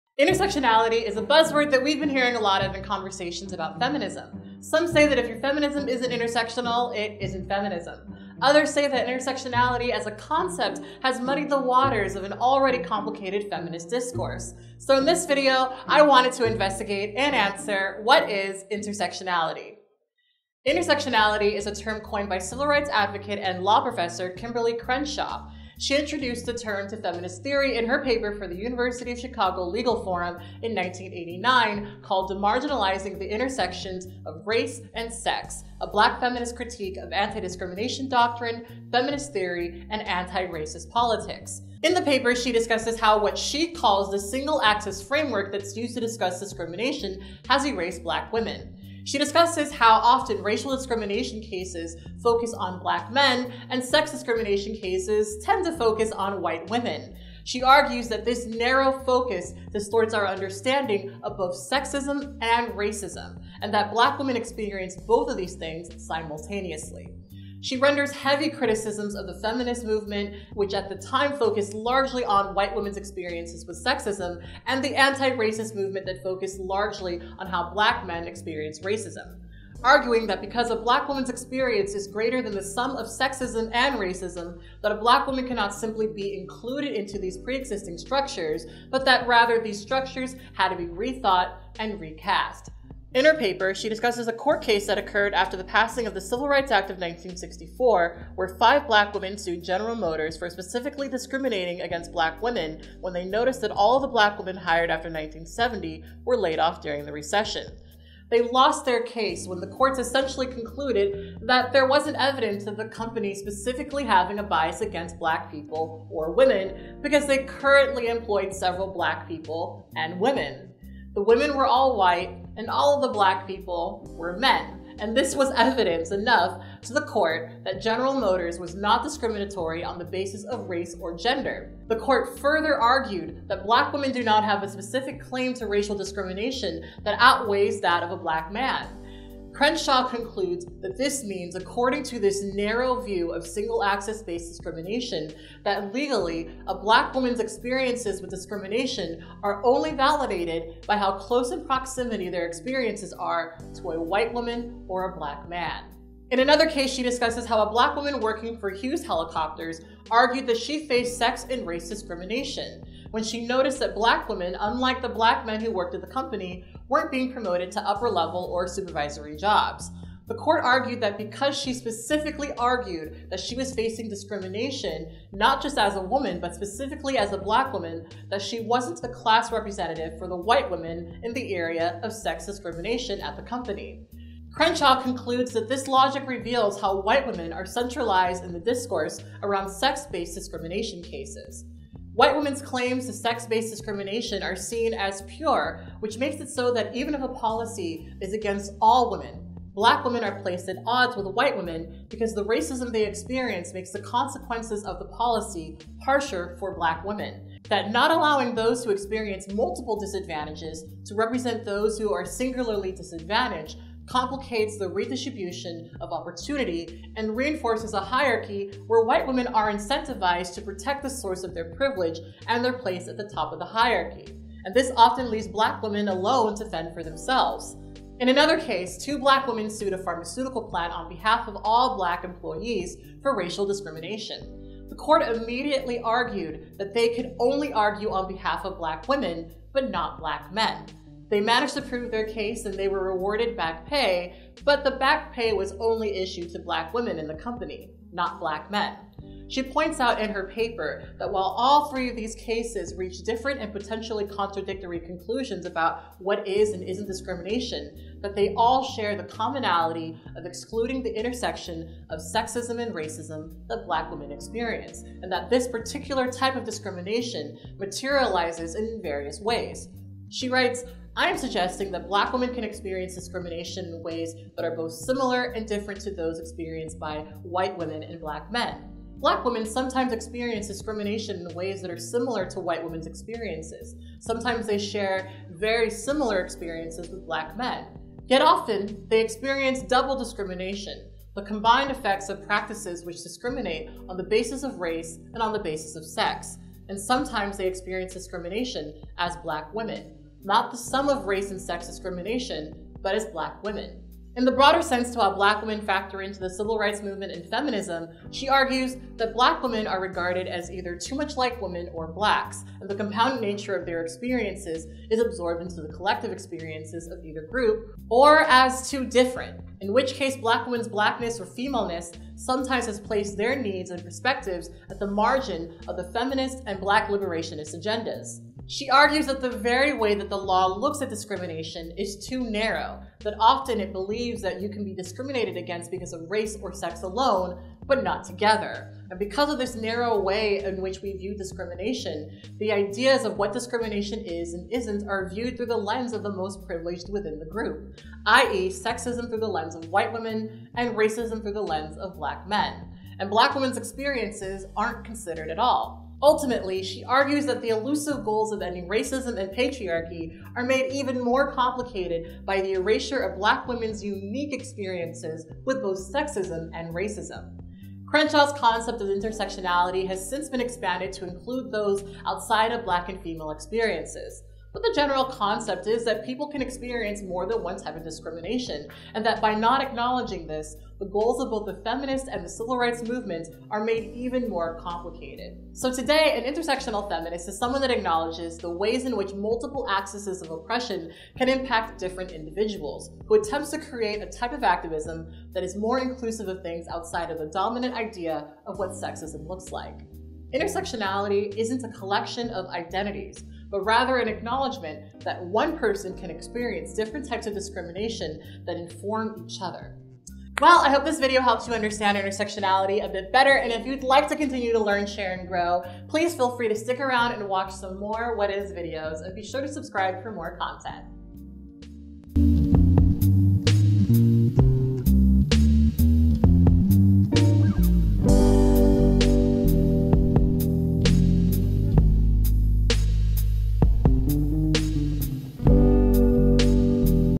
What Is: Intersectionality
You've heard of intersectionality, but where did it come from and what does it mean?

Click Below for More Info vvvvvvvvvvvvvvvvv